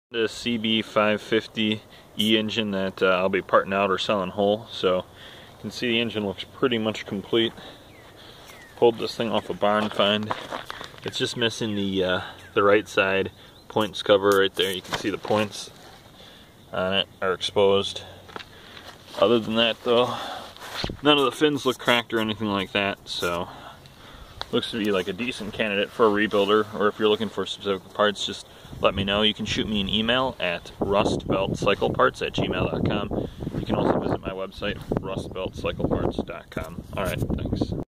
Honda CB550 Engine - For Sale
The whole engine would need to be picked up in Milwaukee, WI.  Parts could be shipped.